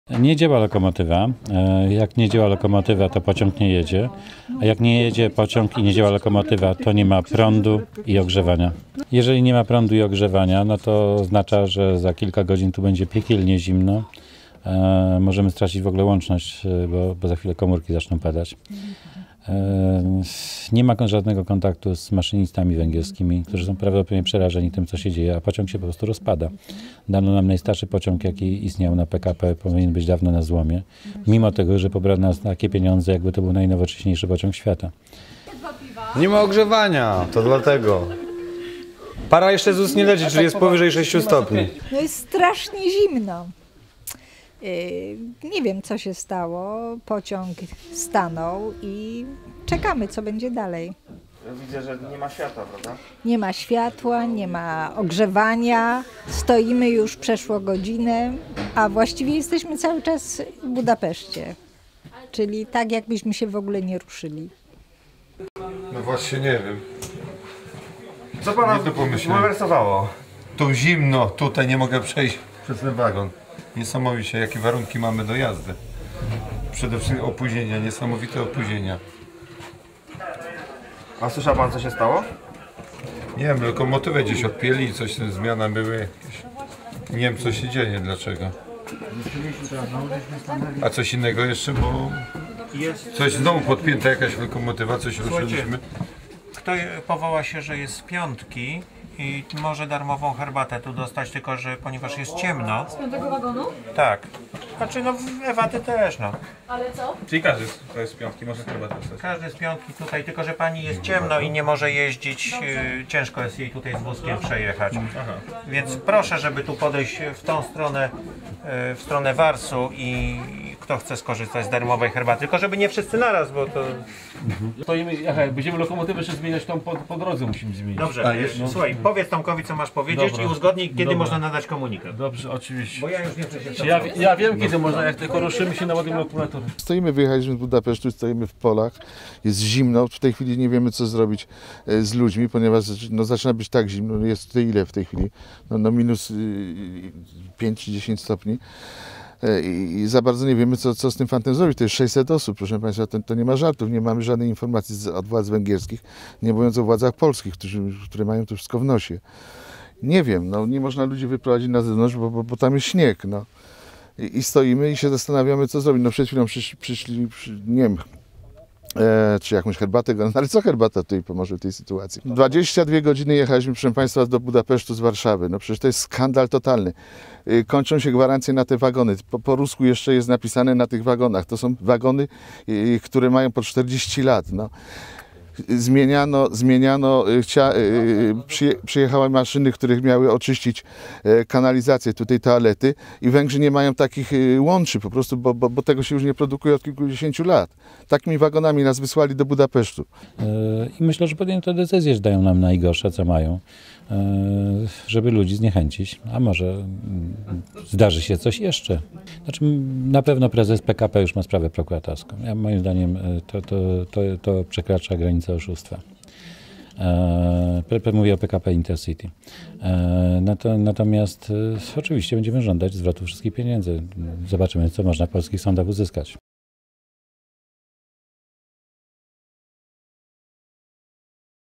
Pociąg horror - PKP do prokuratury!
W wyniku awarii lokomotywy uczestnicy II Wielkiego Wyjazdu na Węgry nie mogli kontynuować podróży powrotnej. Wracając do Polski utknęli pod Budapesztem. Gdy tylko pociąg wyjechał z Budapesztu, awarii uległa lokomotywa. Ze względu na niekorzystne warunki atmosferyczne, a przede wszystkim z powodu dużego mrozu ruch był sparaliżowany.
 Jednym ze skutków awarii lokomotywy był poważny problem z zasilaniem, a przede wszystkim brak ogrzewania. Uczestnicy Wielkiego Wyjazdu na Węgry musieli całą noc czekać aż zostanie podstawiona nowa lokomotywa.
 "Przez całą noc nie było ogrzewania. Pociąg miał 6 godzin opóźnienia. W niektórych wagonach po tym jak wyruszyliśmy nadal nie było ogrzewania a w kilku wagonach padły systemy hamulcowe i trzeba je było wyłączyć. Jesteśmy zmęczeni i wykończeni - relacjonowała na bieżąco Katarzyna Pawlak, dziennikarka GPC, jadąca pociągiem z Węgier.
 "Jechała też z nami też dziewczyna w 8 miesiącu ciąży. Ktoś z innego wagonu chciał dać jej swój koc, który stał się dobrem mocno deficytowym. Konduktor przyłapał przemytnika na gorącym uczynku. Jak mi tłumaczył: zgodnie z przepisami ilość kocy i pościeli w każdym wagonie musi się zgadzać i nie można ich roznosić swobodnie po pociągu. Doszło do kuriozalnej sytuacji, w której trzeba było przemycać koce i się z nimi przemykać przez zimny i ciemny pociąg. Gdybym tego nie widziała na własne oczy ciężko byłoby mi w to uwierzyć. Kompletny absurd dorównujący narracją dziełom Mrożka czy Bareii. Tylko nikomu nie było do śmiechu" - mówiła Katarzyna Pawlak.
 "Sprawa trafi do prokuratury. PKP zapewniło warunki niebezpieczne dla podróżujących" -- zapowiedział Ryszard Kapuściński prezes klubów „GP" .
 "Dwa wagony nie mają hamulców, zaczęły się palić. Musieliśmy podjąć decyzję, czy siedzimy tam pod Budapesztem i zamarzniemy, czy jedziemy bez hamulców. Kolejarze podjęli decyzję, że jedziemy. Niektóre wagony miały o dwa tygodnie przedłużoną gwarancję, po to tylko żeby je podpiąć i mogły jechać do Budapesztu. Po powrocie mają być zezłomowane. Będziemy domagać się zwrotu pieniędzy. Zgłosimy również tą sprawę do prokuratury" --powiedział portalowi niezalezna.pl Ryszard Kapuściński.